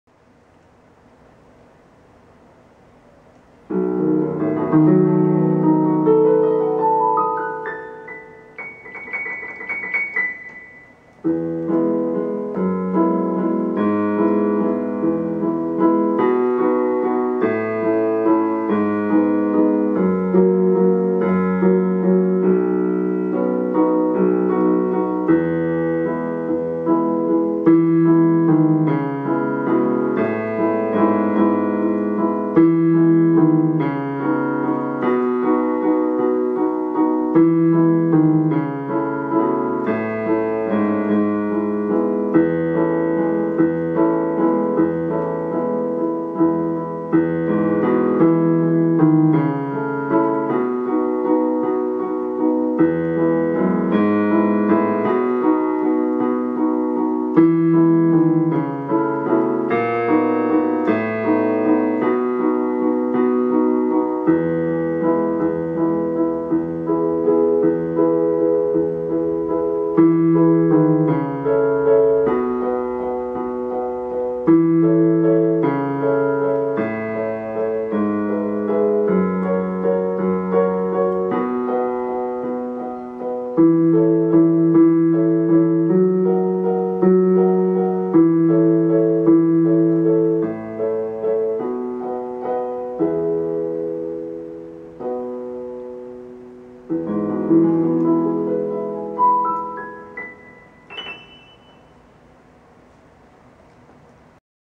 E Major Keyboard Improv
This year I am going around the circle of 5ths to guide my improvisations:

January - key of C Major
February - key of G Major
March - key of D Major
April - key of A Major
May - key of E Major

This month I switched things up and had the melody in the left hand. I hope you enjoy!